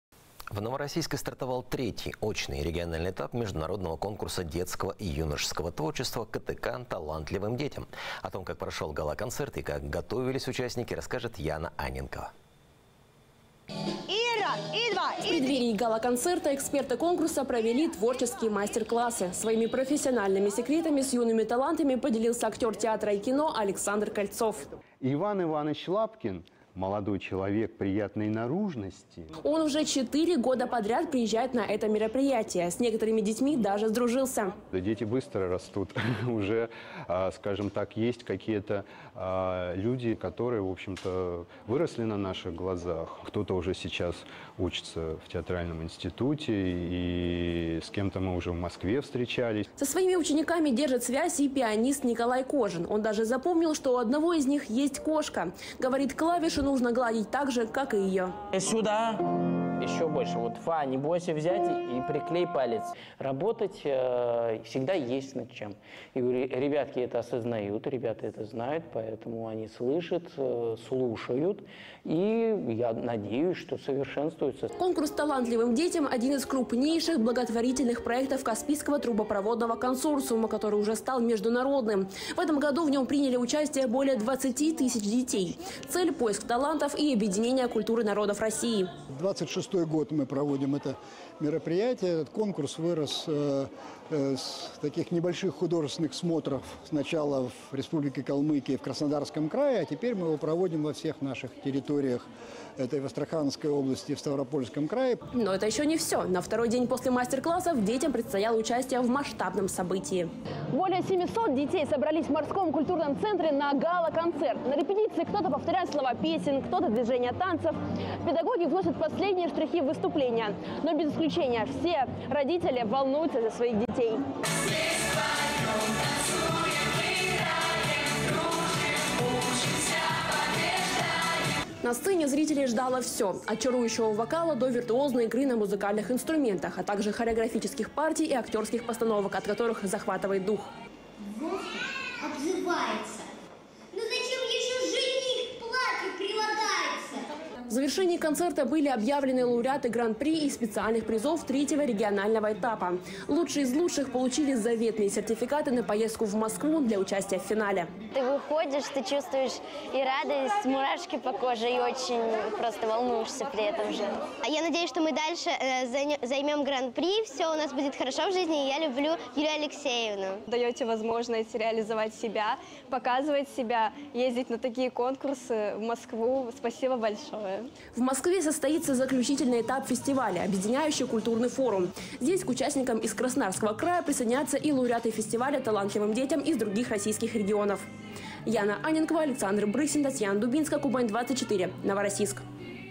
III региональный этап конкурса "КТК - талантливым детям" в Новороссийске
Репортаж телеканала "Кубань 24" о мастер-классах и гала-концерте, состоявшемся 2 октября 2024 года